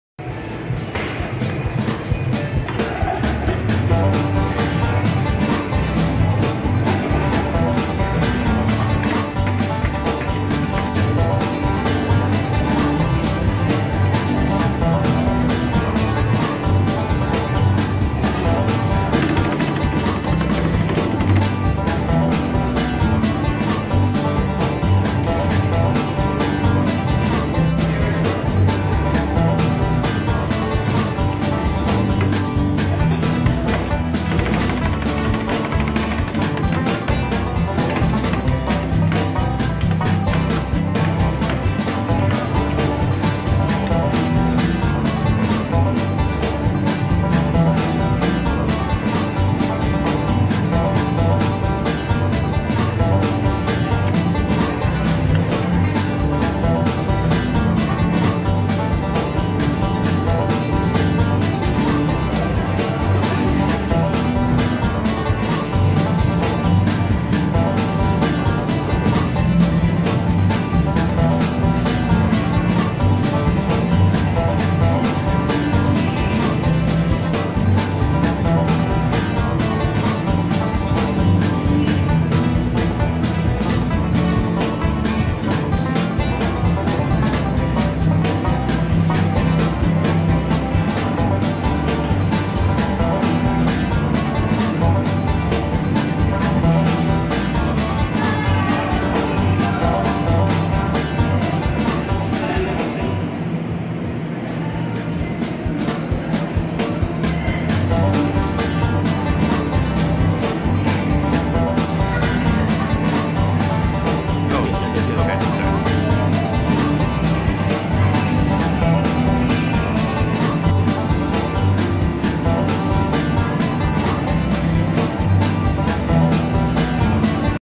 The Abita Butter Beer Wizard Rolled IceCream
Local beer was used to make this Abita Butter Beer Wizard handcrafted ice cream roll from Sweet Rolls Rolled Ice Cream.